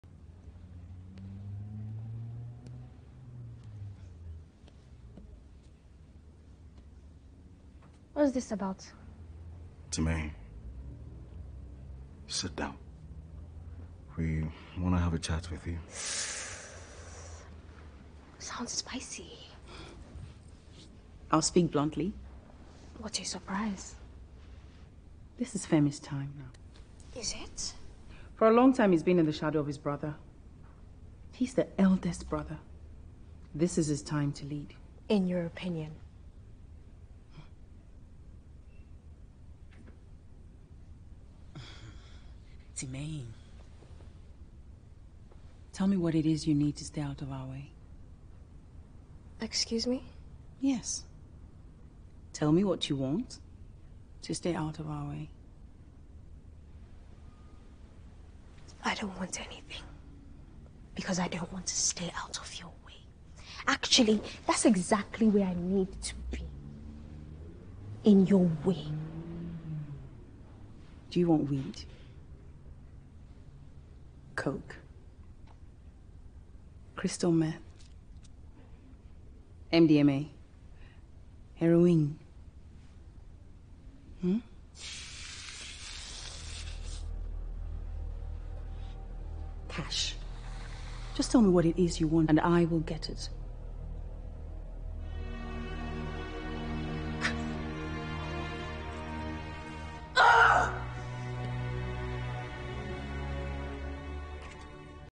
Played Timiyen, Olayinka and Femi from the movie Blood Sisters
Hey.